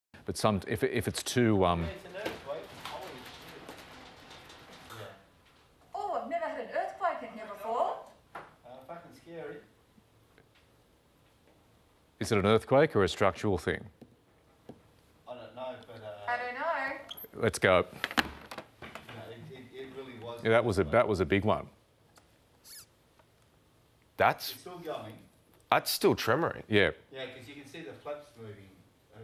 ABC stüdyosunda kaydedilen Melbourne deprem.
Güneydoğu Avustralya sakinleri, Çarşamba sabahı saat 9.15 civarında Melbourne ve Victorianın kırsal bölgeleri boyunca dalgalanan, Canberra, Adelaide ve NSW'in bazı bölgelerine kadar ulaşan bir depremle şok yaşadı.